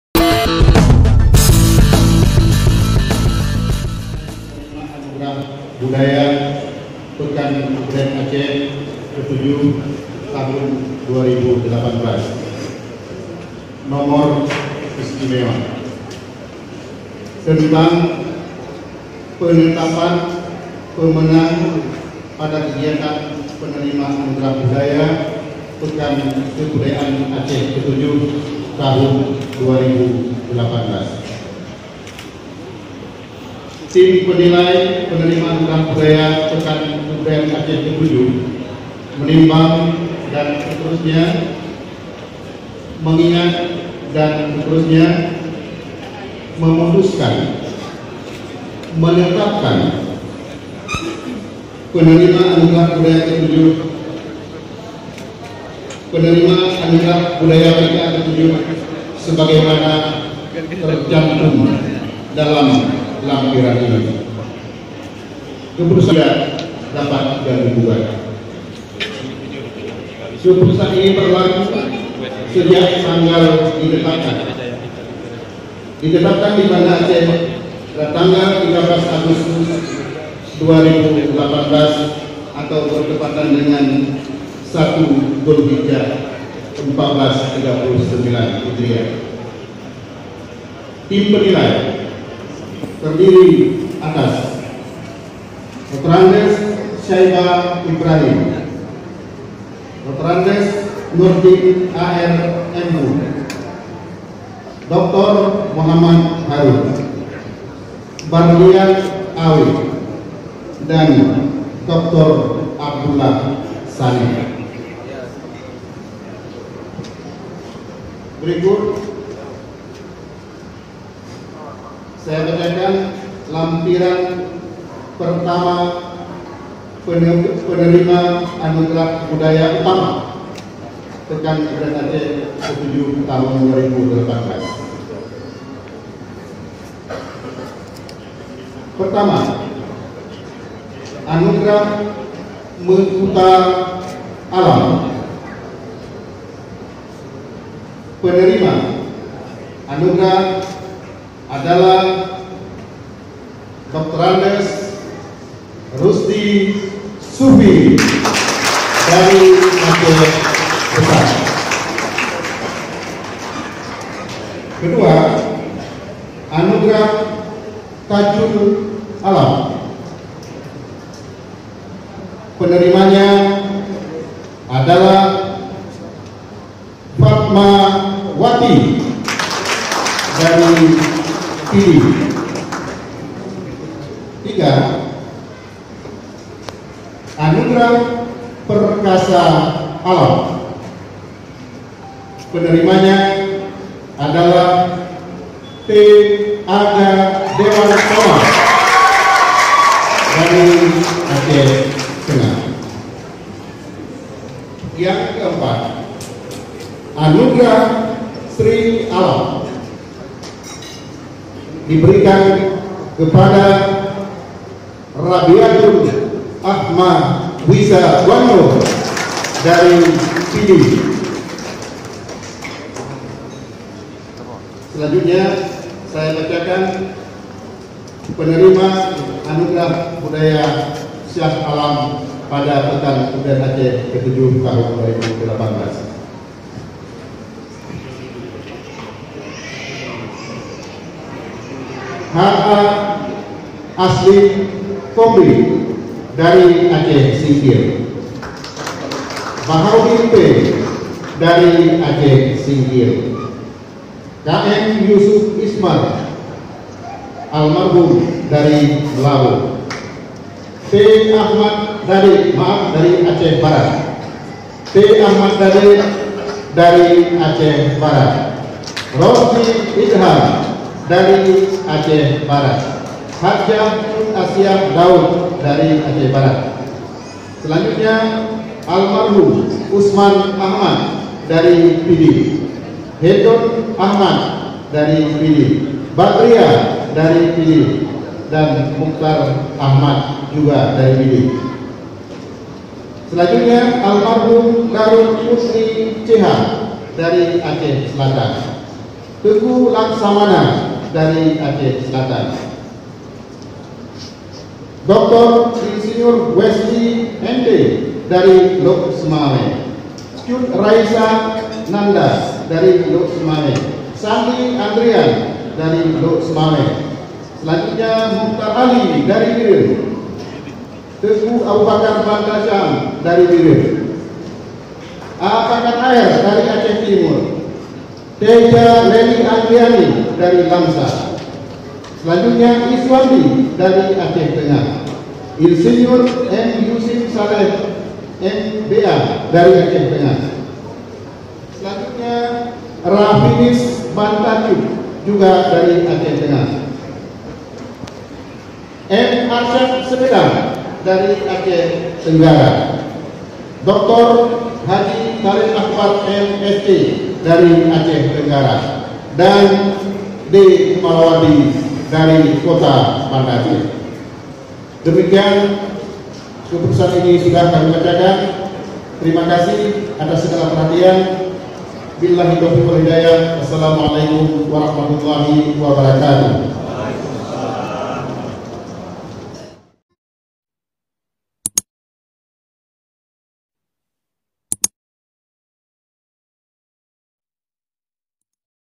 Inilah Orang Orang Hebat Penerima Anugrah Budaya Aceh
Inilah nama nama penerima Anugrah Budaya  di PKA-7 2018 meliputi kategori Meukuta Alam,  Tajul Alam,  Perkasa Alam, Sri Alam dan Shah Alam.